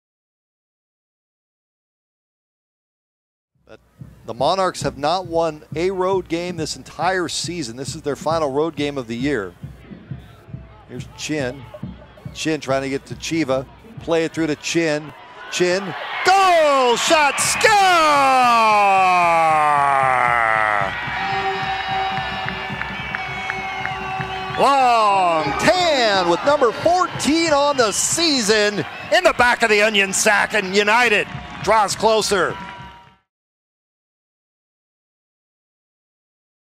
GOAL: Long Tan pulls Arizona United SC within one
With Arizona United down two, Long Tan scored his 14th goal of the season to pull the club within one.